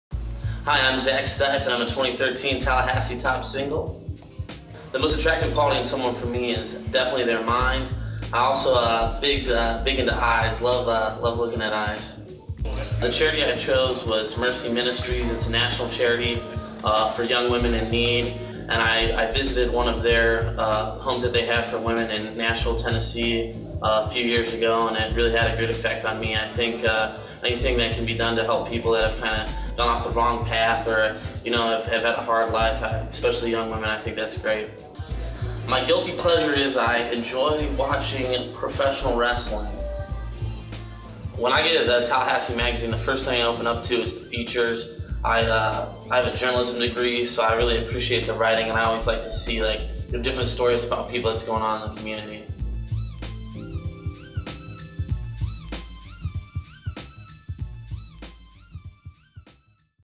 2013 Tallahassee Top Single: Zach Stipe
Whether you're currently single, married or just want to support local charities, Tallahassee Top Singles, starting at 7 p.m. Saturday, July 20, at Hotel Duval, will be the event of the summer. Eighteen of Tallahassee's most eligible men and women have agreed to be auctioned off at the fourth annual Top Singles event. Winning bidders from each auction receive a lunch date with his or her single and each winning bid will be donated to the single's charity of choice.

Videography & Editing: Carlin Trammel